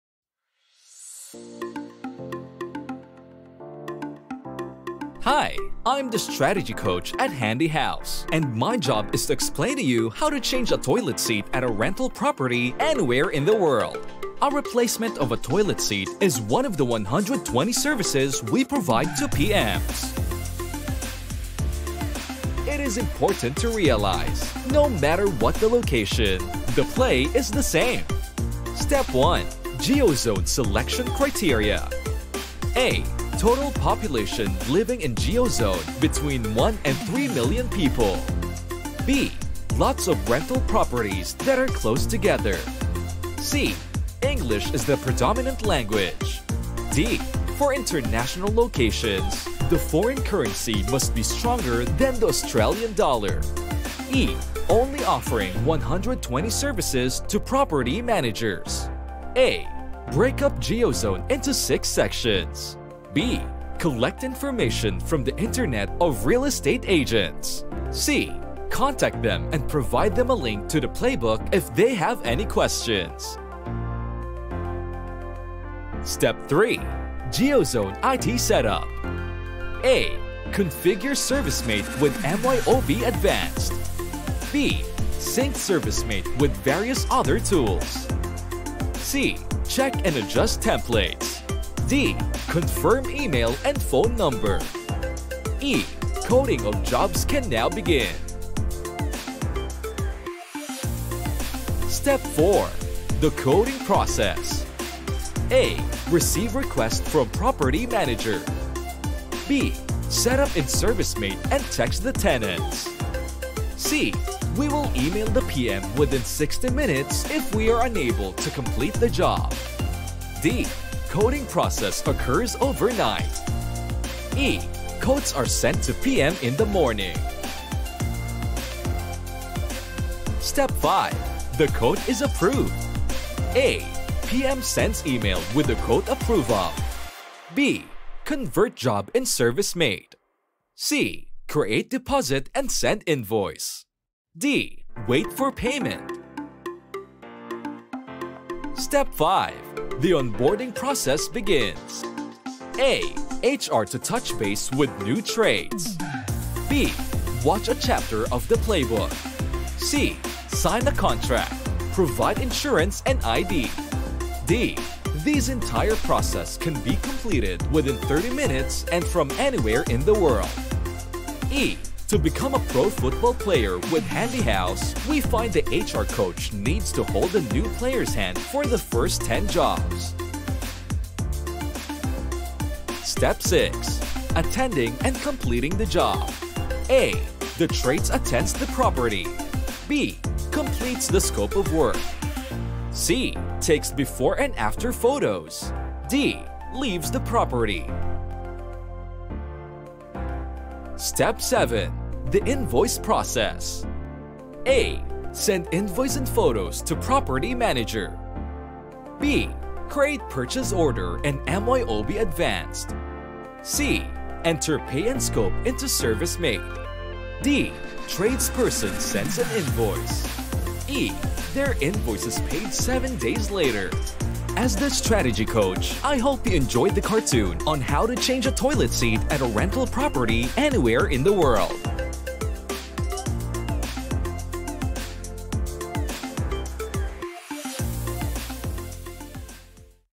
The Kick-Off - How To Expand to New Geo Zone
The Kick-Off Definition
 - Kick-offs are used to start each half of the match or period of extra time. Restart kicks are used to resume play. All kick-offs and restart kicks are drop kicks. Sanction: The non-kicking team has the option of the kick being retaken or a scrum.

The Strategy Coach says that you should:
Watch this cartoon if you are a Coach
And...
Wanting to know the process of expanding to a new Geo Zone.